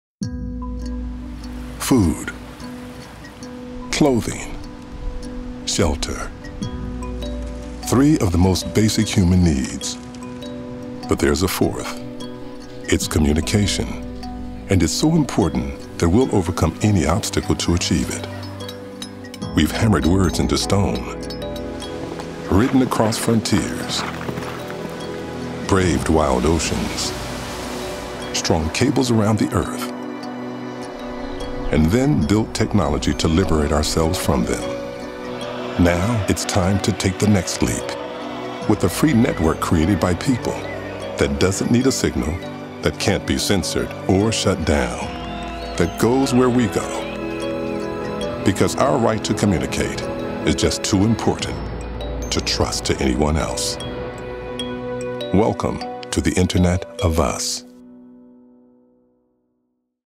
FireChat Internet of us | WIRED
FireChat: the smartphone app that doesn't need a signal, can't be censored or shut down